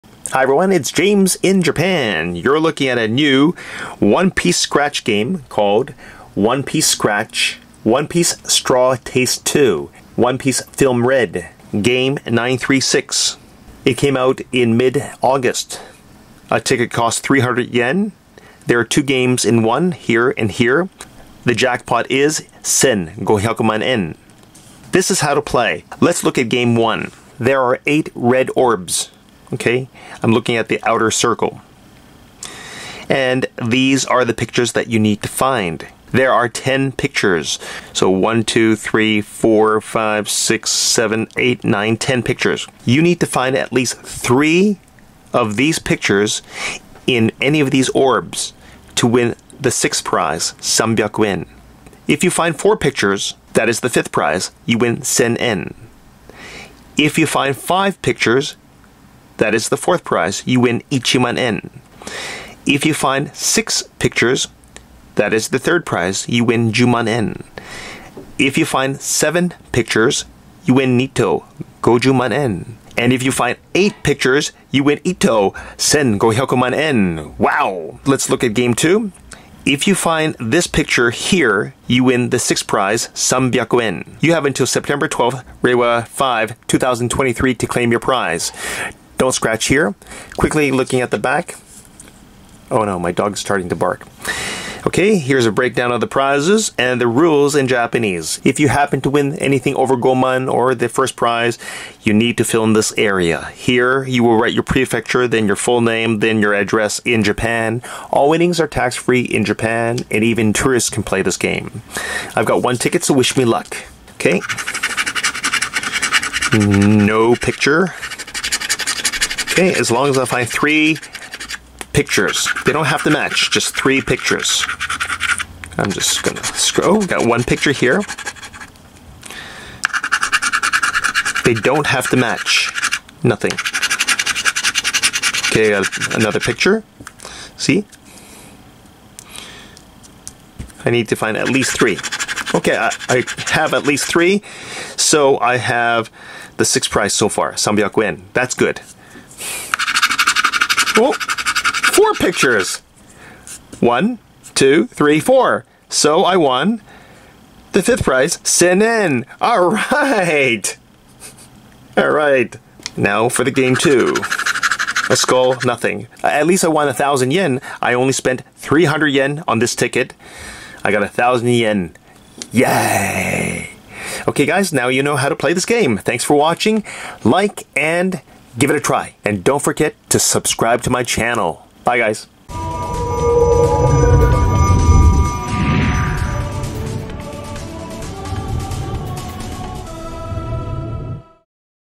How to play ONE PIECE 936-2gamesin1 -SEE HOW MUCH I WON!
ワンピーススクラッチ 麦わらの一味2 ONE PIECE FILM RED

こちらの発売期間は

2022/8/17～2022/9/13

換金期間は

2023/9/12まで